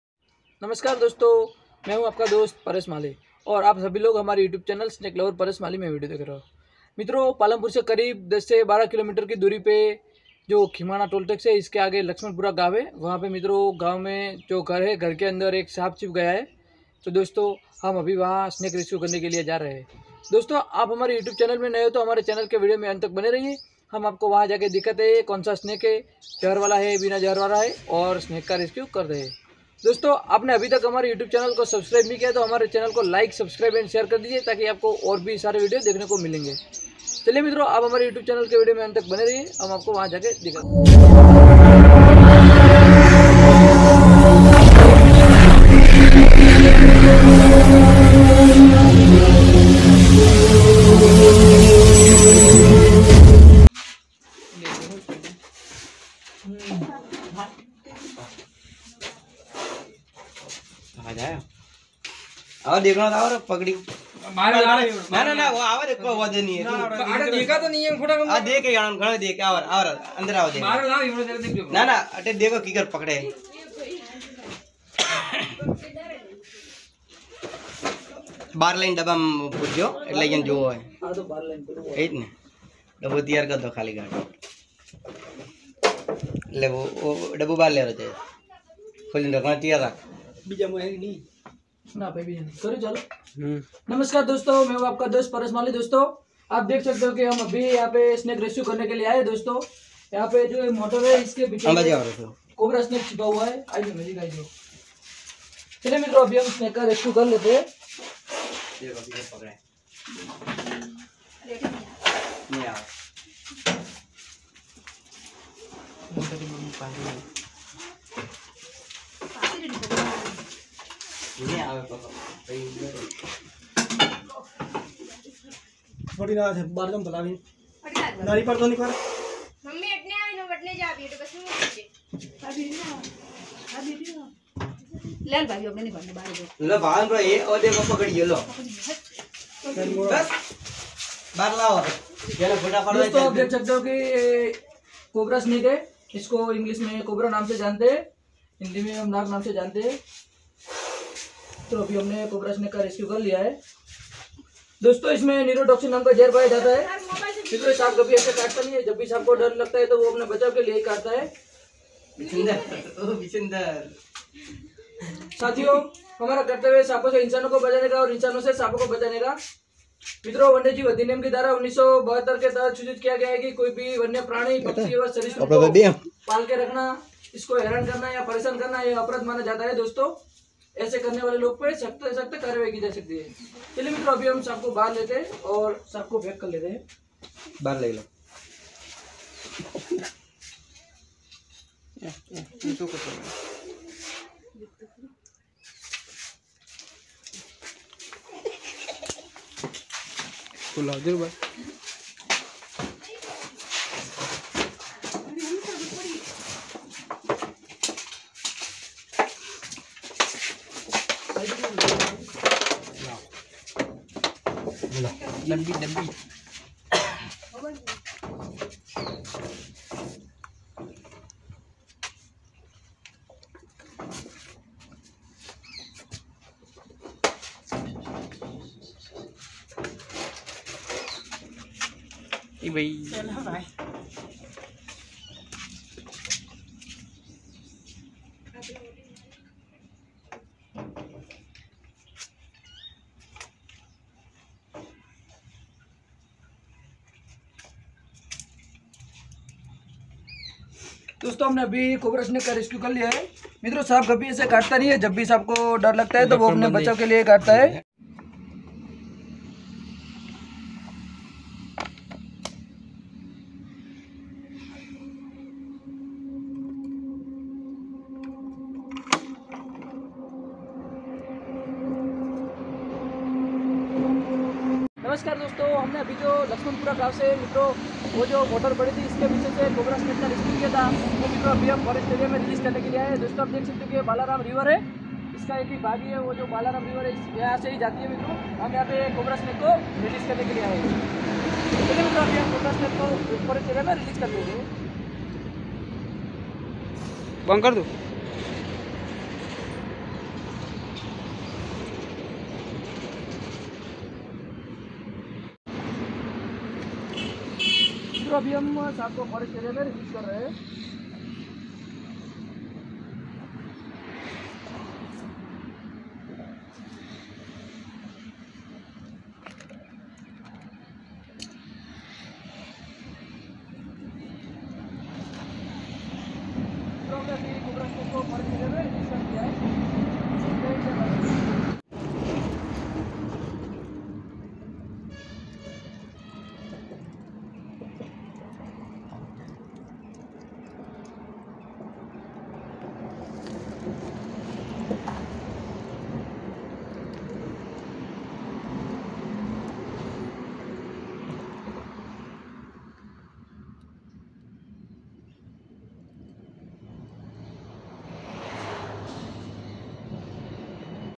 देखिए आखिर जहरीला नाग कहां छुपा बैठा था आखिरी आज पकड़ में आ ही गया कैसे क्या हुआ। MO:7487847827#like
सद्भावना फाउंडेशन बनासकांठा पालनपुर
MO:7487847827
स्नेक लवर परेश माली
🐍🚨 24 HOUR RESCUE TEAM 🚨🐍